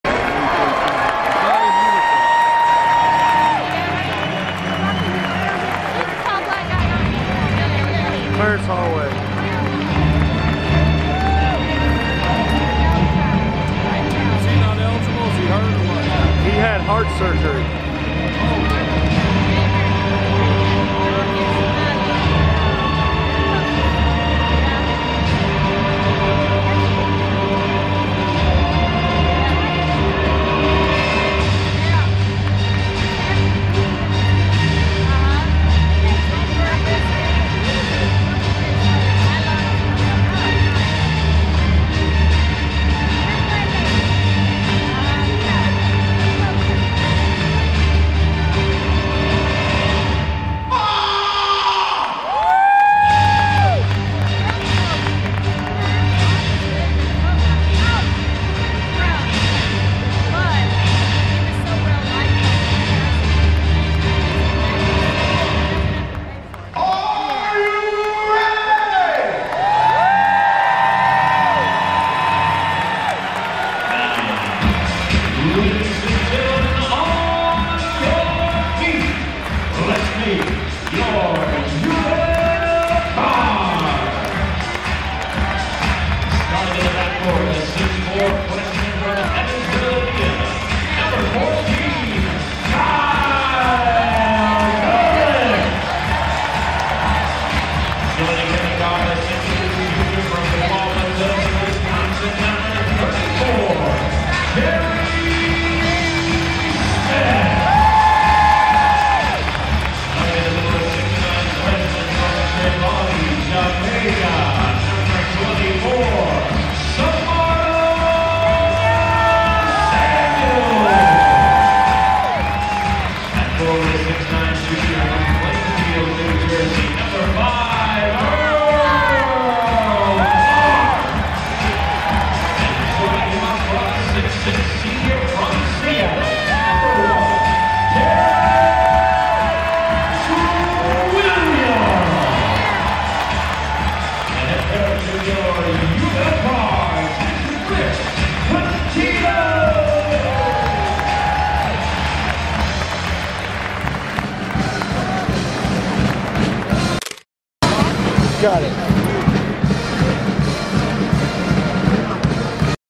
Louisville Basketball Intro Video vs Indiana St.divx
Here is the awesome new starting lineup intro video for the 2008-09 Louisville Cardinal Basketball team.